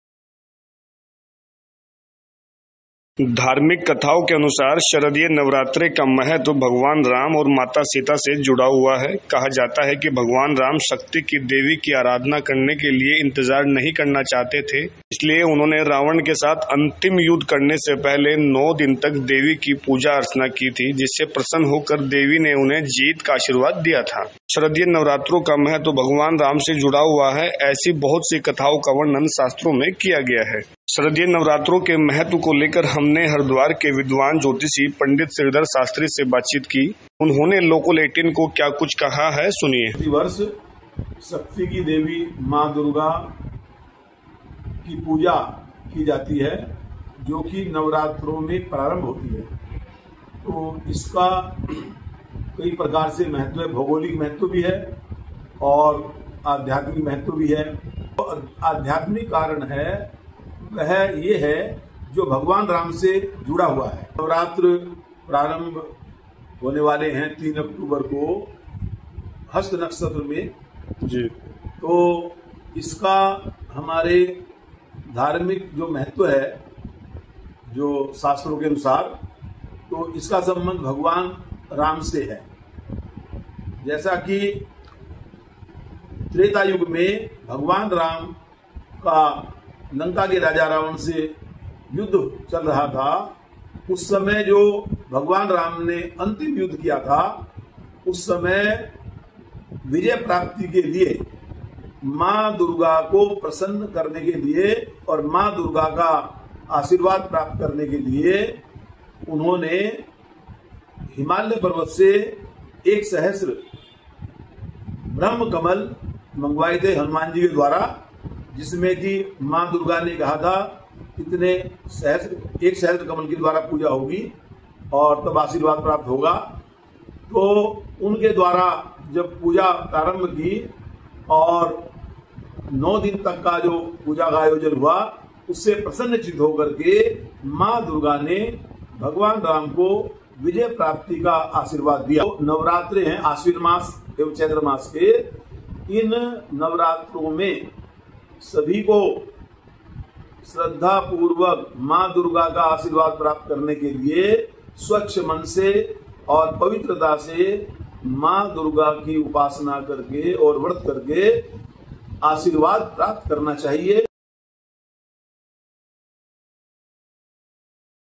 Shardiya Navratri 2024: Maa Durga की उपासना से कैसे पाएं मनचाहा फल और विजय
Navratri days are considered extremely special and significant for the worship and recitation of the Goddess of Power. In 2024, Navratri will begin on the first day of the bright fortnight of the month of Ashwin, which is October 3rd, and will continue till the ninth day, October 11th. Although Navratri occurs four times a year, Chaitra and Sharad Navratri are considered the most important. During Navratri, fasting, worship, and recitation for Goddess Durga are considered significant.
शक्ति की देवी की उपासना पूजा पाठ करने के लिए नवरात्रि के दिन बेहद ही खास और महत्वपूर्ण बताए गए हैं. साल 2024 में नवरात्रि आश्विन मास शुक्ला पक्ष की प्रतिपदा 3 अक्टूबर से प्रारंभ हो जाएंगे जो नवमी 11 अक्टूबर तक किए जाने का विधान बताया गया हैं. साल में नवरात्र चार बार होते हैं लेकिन इनमें चैत्र और शारदीय नवरात्रि ही प्रमुख बताए गए हैं. नवरात्रों में देवी दुर्गा के निमित्त व्रत, पूजा पाठ आदि करने का महत्व बताया गया है.